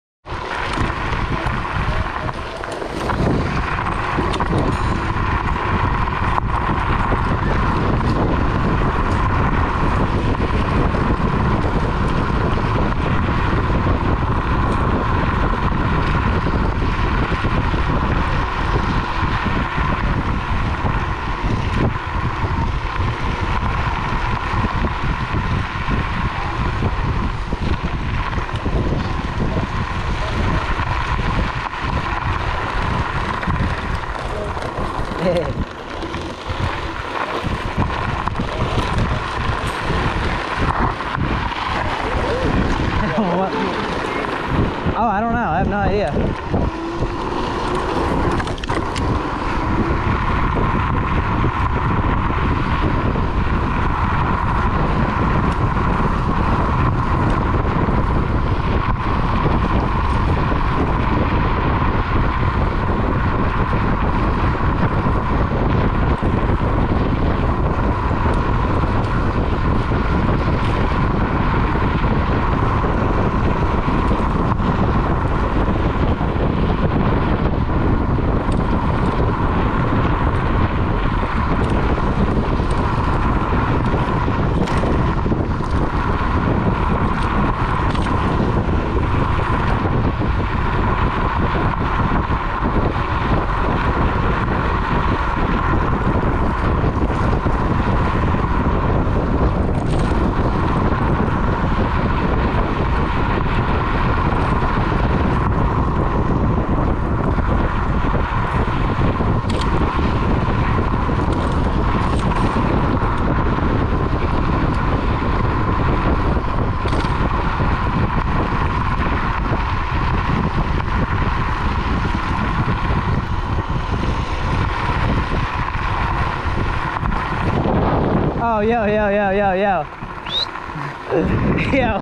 great ride up the river
riding with big mike and zeezo